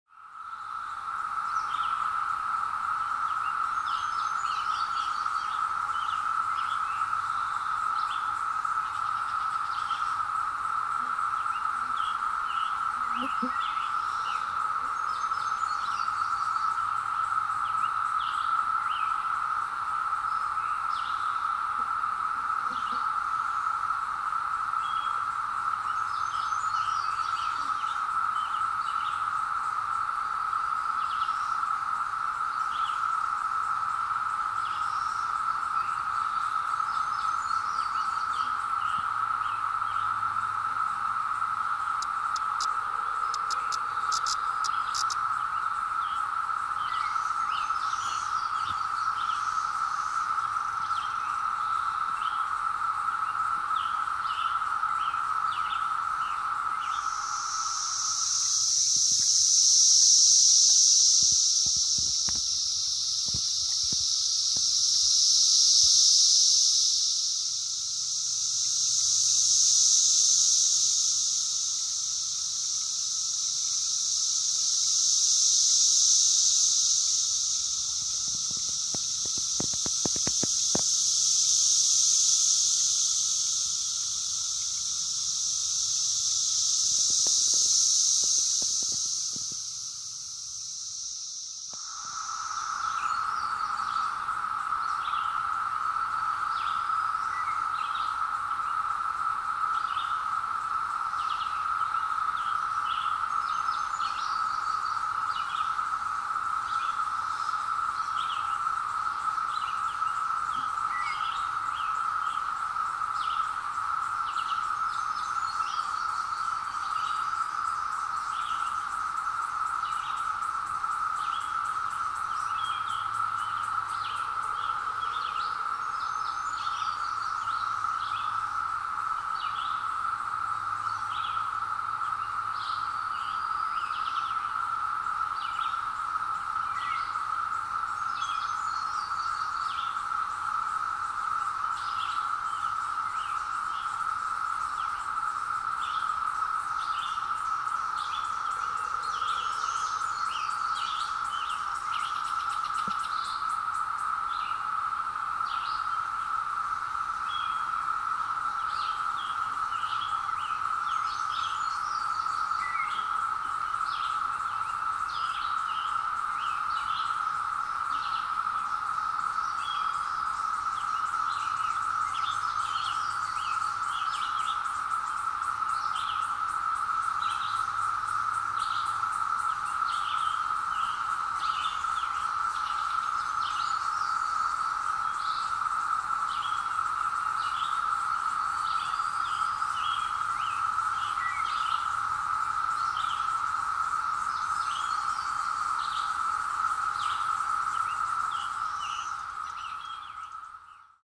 Cicadas Arrive After 17 Years | Sounds | Pictures
Wanted to share with you this interesting creature. They are so friendly and really do not fly away from you. The sound at times is like a wave across the woods. Yet, if your are in a low canopy, it can be quite loud. I enclosed some pictures and and the hill sound was done with a Zoom H2. - I hope you enjoy.

#17 Year Cicada
Cicadas Arrive
Steve Addis